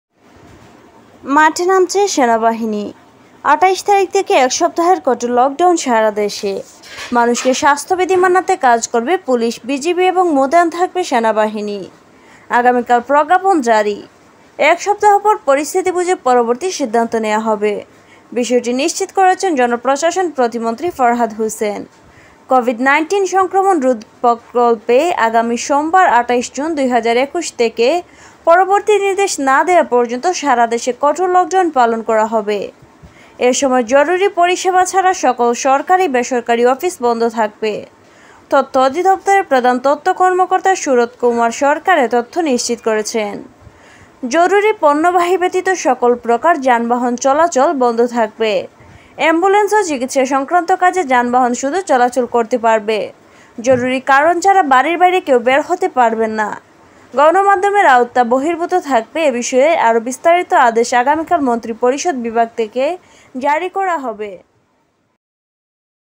মাঠে নামছে সেনাবাহিনী,সাবধান হউ জনতা
মাঠে নামছে সেনাবাহিনী
সিলেটভিউ ডেস্ক
প্রকাশিত : ২৫ জুন, ২০২১ ২১:২৩

২৮ তারিখ থেকে এক সপ্তাহের কঠোর লকডাউন সারাদেশে। মানুষকে স্বাস্থ্যবিধি মানাতে কাজ করবে পুলিশ, বিজিবি এবং মোতায়েন থাকবে সেনাবাহিনী। আগামীকাল প্রজ্ঞাপন জারি। এক সপ্তাহ পর পরিস্থিতি বুঝে পরবর্তী সিদ্ধান্ত নেয়া হবে।

বিষয়টি নিশ্চিত করেছেন জনপ্রশাসন প্রতিমন্ত্রী ফরহাদ হোসেন।

কোভিড ১৯ সংক্রমণ রোধকলপে আগামী সোমবার ২৮ জুন ২০২১ থেকে পরবর্তী নির্র্দেশ না দেয়া পর্যন্ত সারাদেশে কঠোর লকডাউন পালন করা হবে।

এ সময় জরুরি পরিষেবা ছাড়া সকল সরকারি বেসরকারি অফিস বন্ধ থাকবে।

তথ্য অধিদপ্তরের প্রধান তথ্য কর্মকর্তা সুরথ কুমার সরকার এ তথ্য নিশ্চিত করেছেন।

জরুরী পন্যবাহী ব্যতীত সকল প্রকার যানবাহন চলাচল বন্ধ থাকবে। অ্যাম্বুলেন্স ও চিকিৎসা সংক্রান্ত কাজে যানবাহন শুধু চলাচল করতে পারবে।
জরুরী কারণ ছাড়া বাড়ির বাইরে কেউ বের হতে পারবেন না।

গণমাধ্যম এর আওতা বহির্ভূত থাকবে।

এ বিষয়ে আরো বিস্তারিত আদেশ আগামীকাল মন্ত্রিপরিষদ বিভাগ থেকে জারি করা হবে